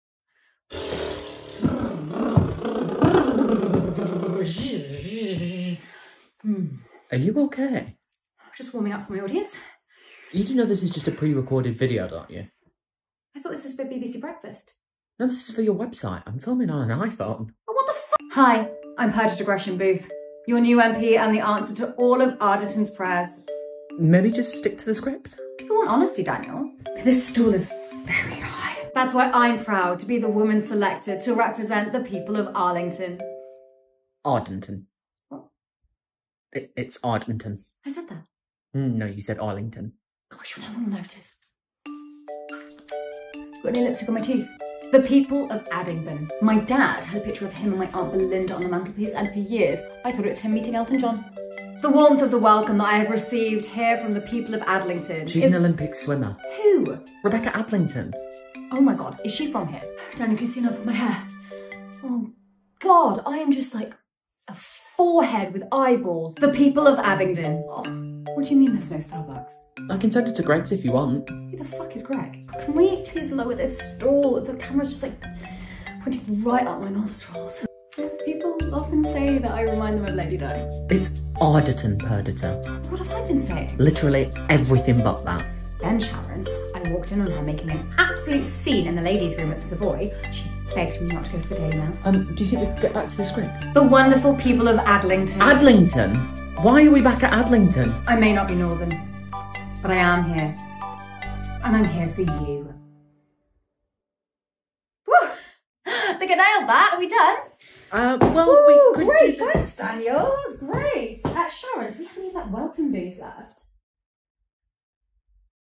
RIGHT DISHONOURABLE FRIEND
OUR FIRST SHOW OF 2023: RIGHT DISHONOURABLE FRIEND!

Right Dishonourable Friend opens at VAULT Festival for its world premiere from the 14th - 18th February at the Cage at 20:00

Written by Phoebe Batteson-Brown and Eoin McKenna
Directed by Kalya Feldman
Supported by Stonewall Housing

Tony Blair, Nick Clegg, Rishi Sunak – She’s hardly the first Southerner to represent a Northern constituency. What she lacks in political experience, she makes up for in… charm?

When nepotism hire of the year, Perdita, is appointed as MP for the fictional Lancashire town of Ardenton.

She thinks that her wealth of social experience and the fact that she’s the former Home Secretary’s Daughter, are all the qualifications that she needs, how wrong she is...